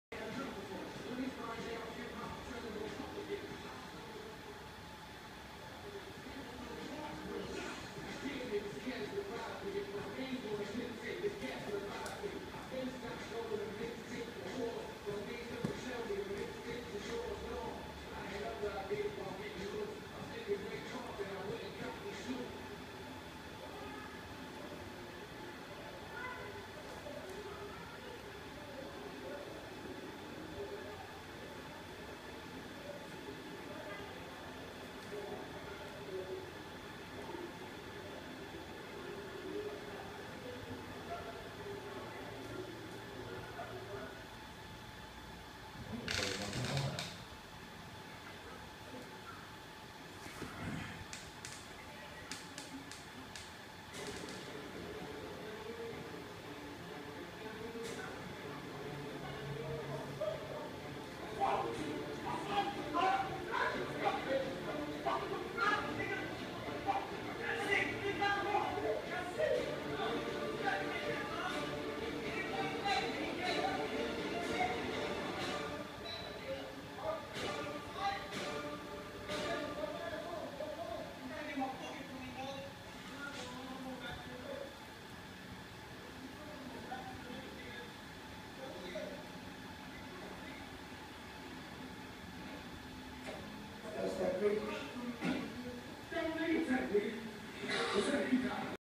Cats playing in new makeshift tent
Cheap way of giving them tent to play with. $2 hamper basket from Daiso. it did not last long unfortunately!